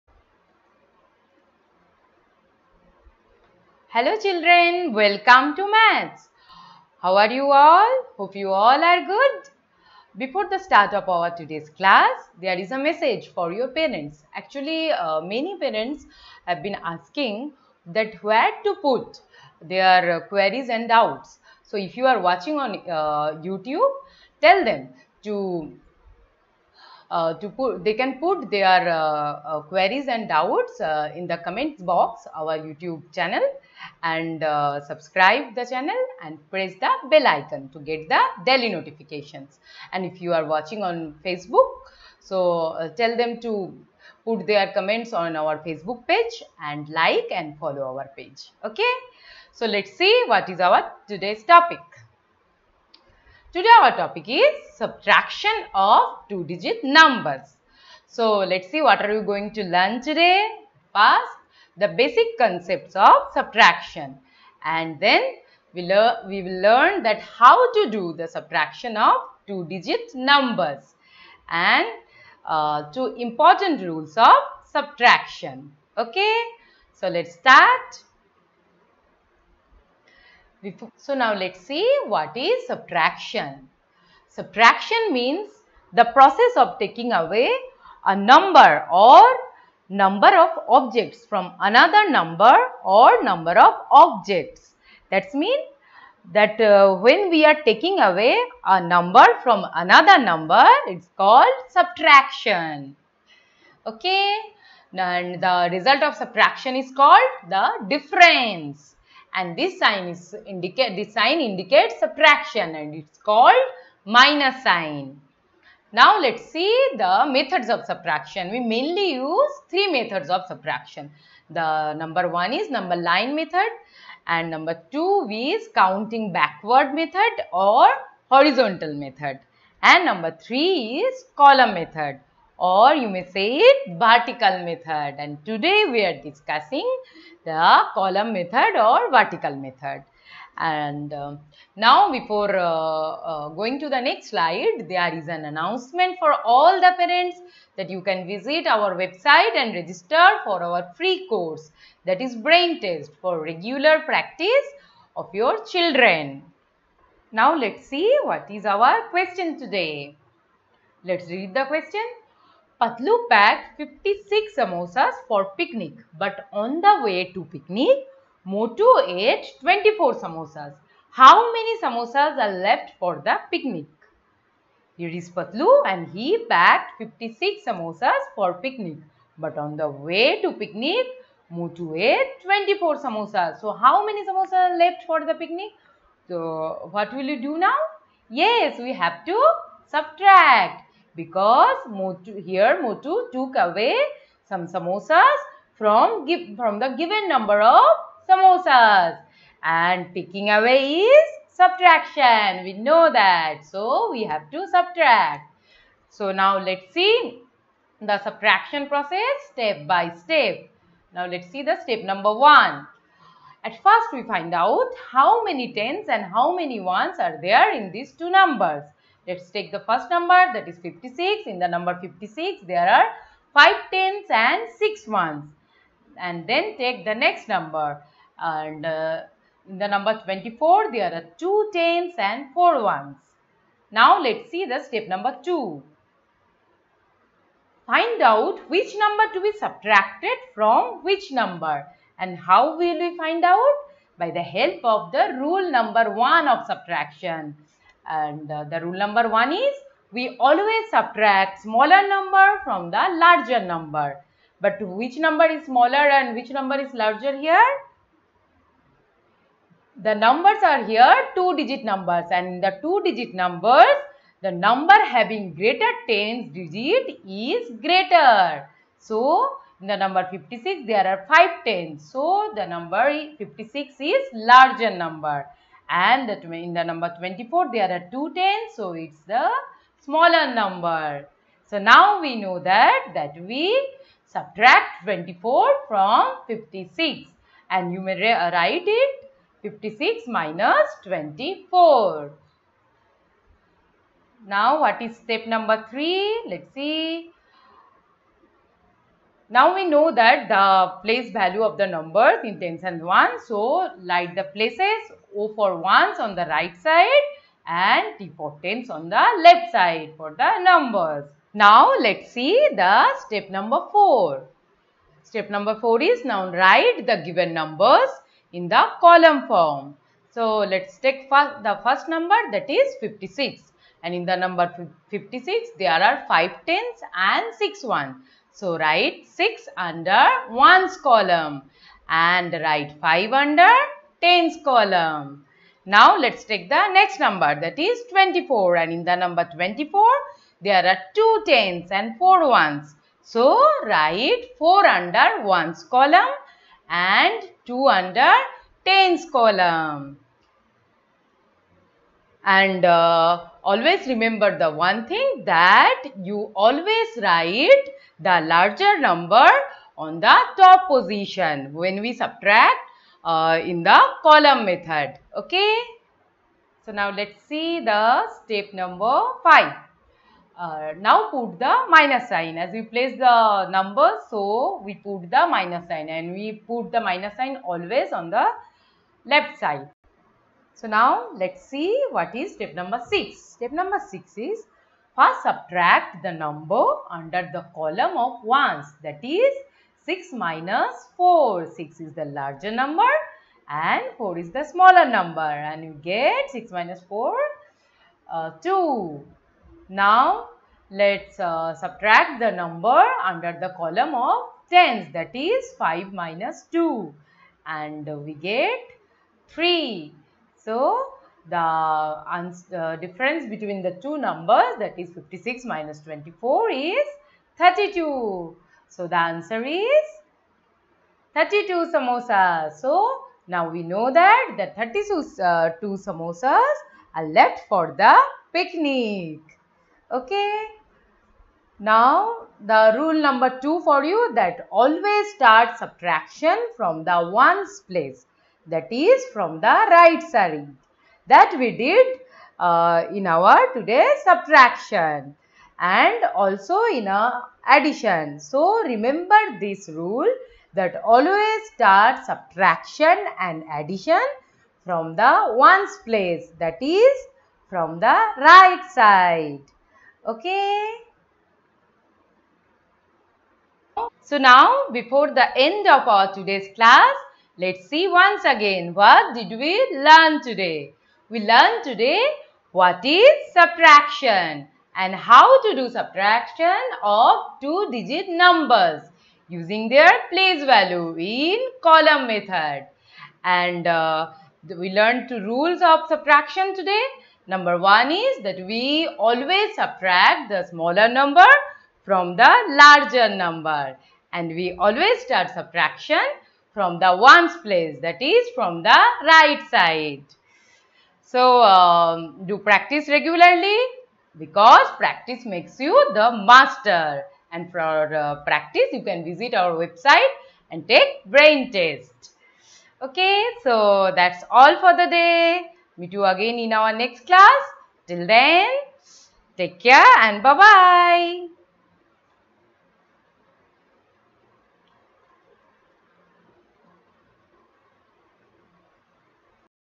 Subtraction of 2-Digit Numbers without borrowing |Class 1 to 5 | Subtraction concepts
By: Swarnali Saha.
For FREE REGULAR PRACTICE.
Do you want to to teach your child the 2 most important rules of Subtraction? Then this Video Lecture is for YOU.
This Video Lecture (Lesson/Class) by Swarnali Saha is about Subtraction of two digit (2-digit) Numbers. This lesson is relevant for all primary school students (Class-1, Class-2, Class-3 and Class-4).